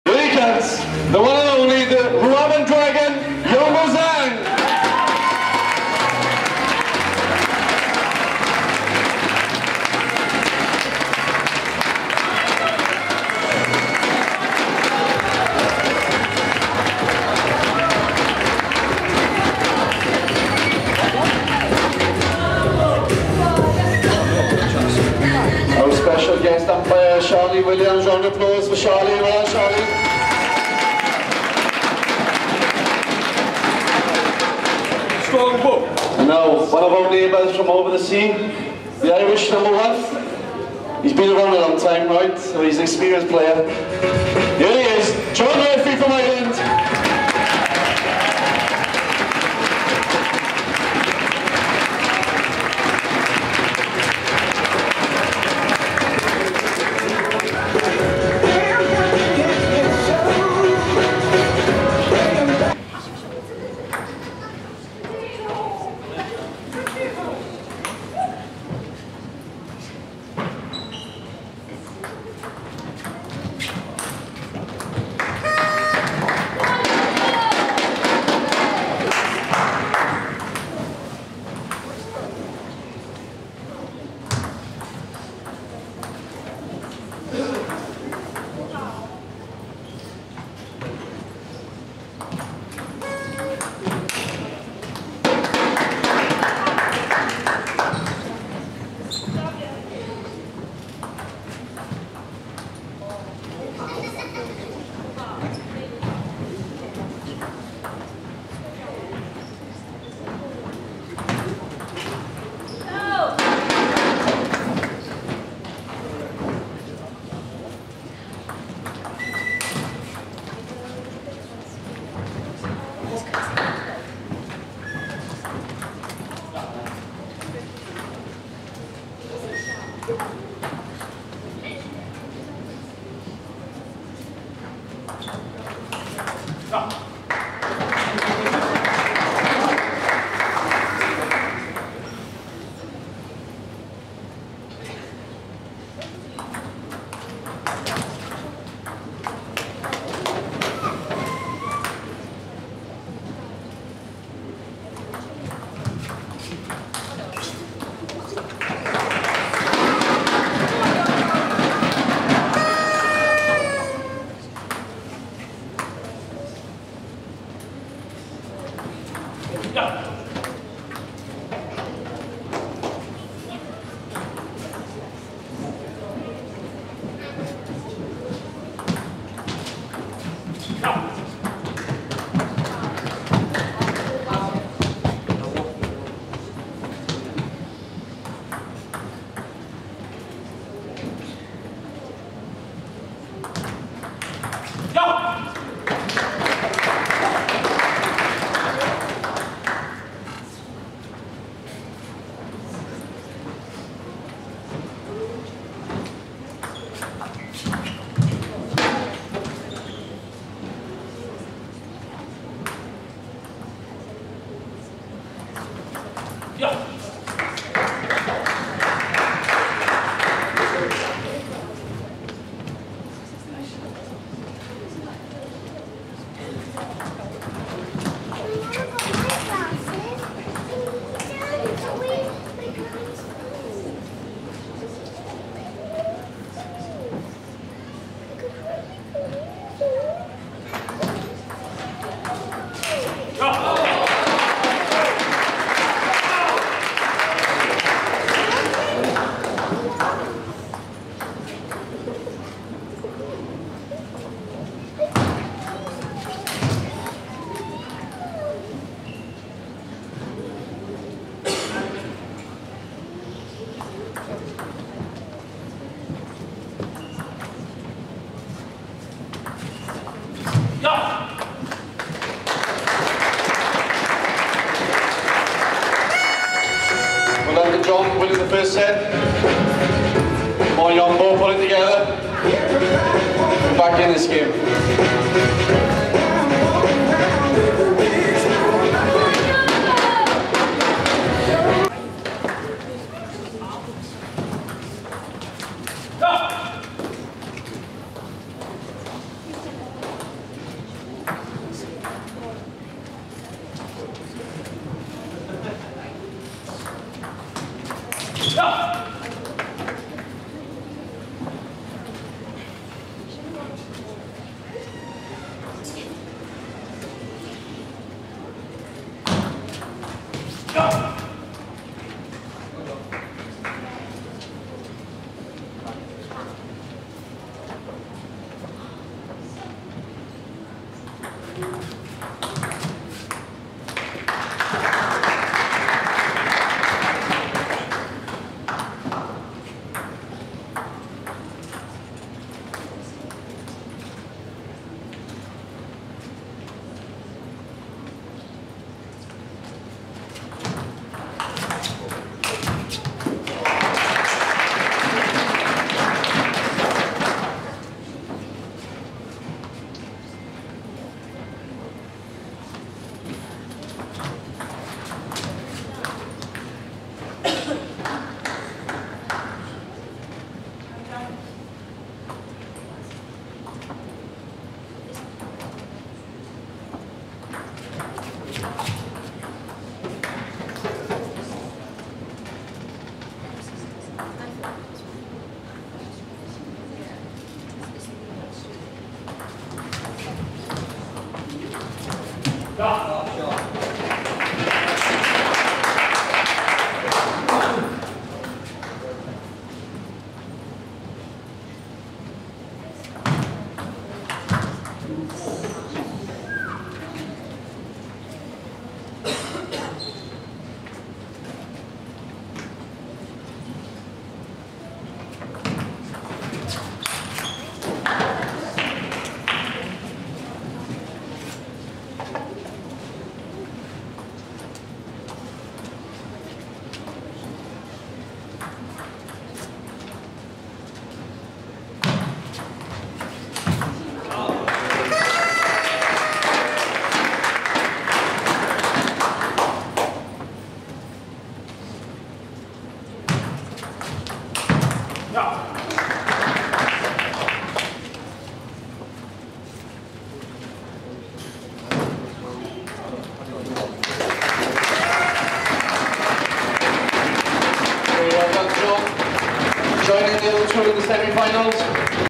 Table Tennis Welsh Masters 2011 (Quarter Final 3)
Yongbo Zhang (China) v John Murphy (Dublin)